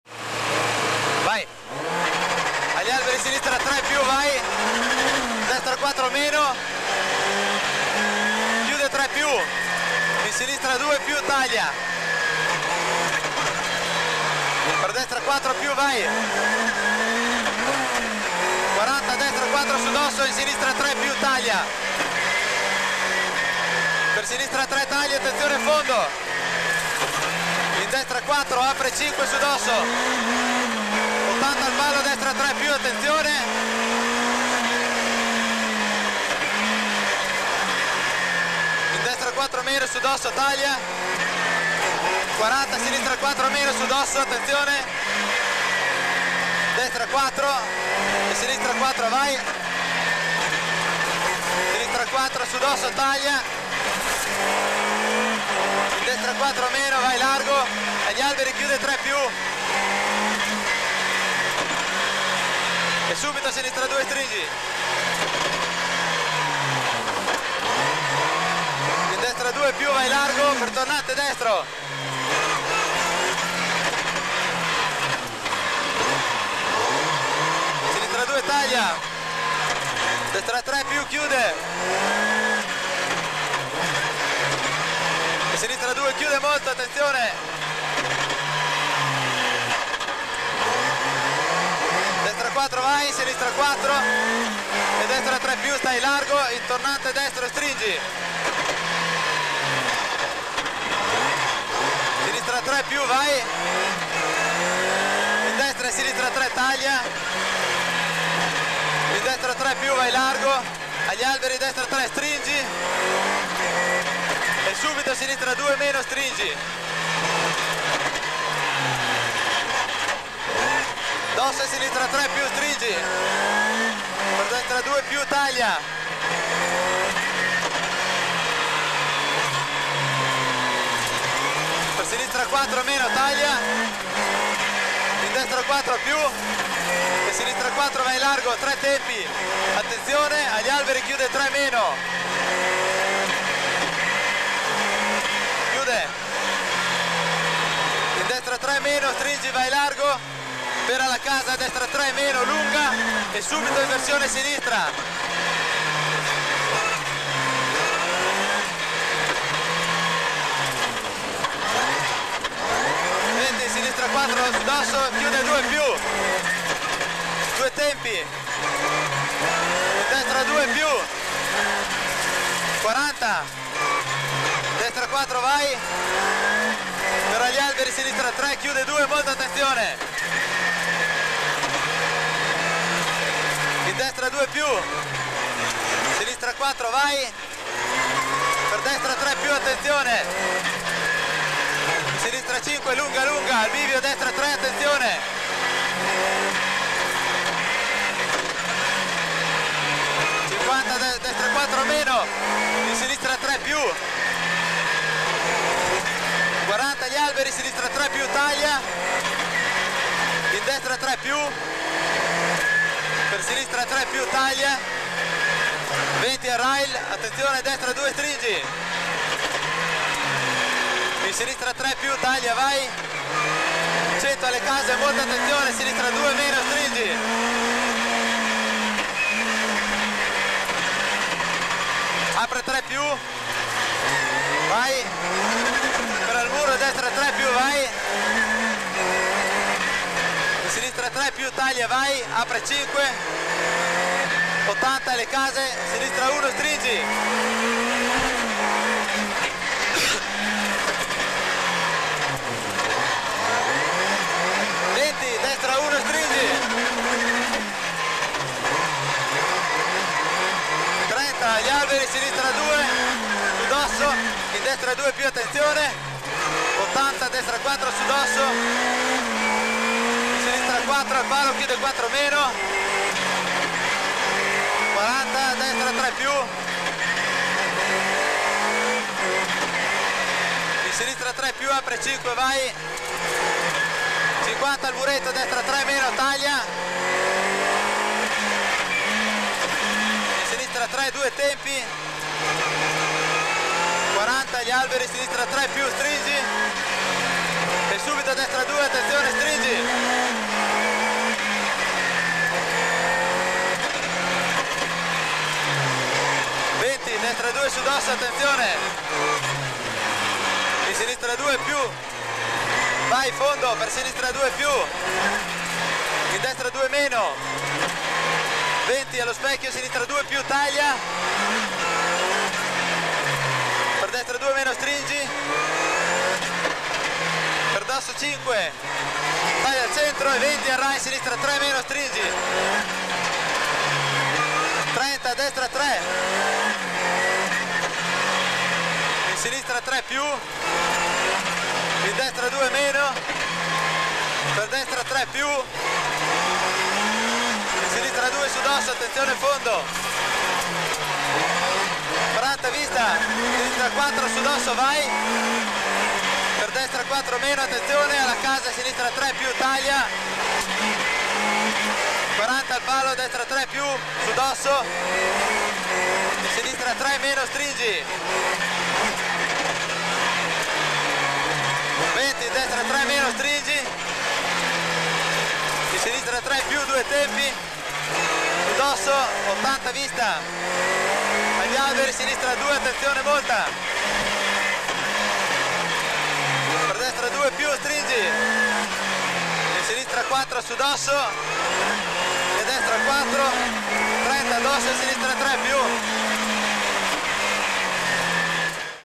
Cameracar Rally Città di Bassano 1992 by Video Si
Saliamo a bordo della BMW M3 dell'equipaggio FIORA-TITA al rally Città di Bassano 1992 buona visione. Video Si